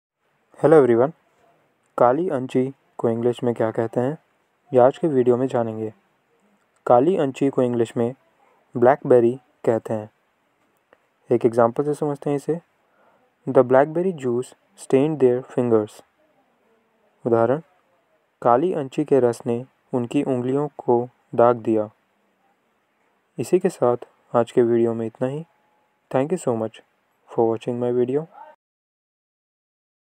Kaali anchi ko english mein kya kahate hain | काली अंची को इंग्लिश में क्या कहते हैं | Blackberry
Kaali anchi ko English me kya kahate hain
Kaali anchi को इंग्लिश में क्या कहते है

काली अंची को इंग्लिश में blackberry कहते हैं
Kaali anchi ko English mein blackberry kahate hain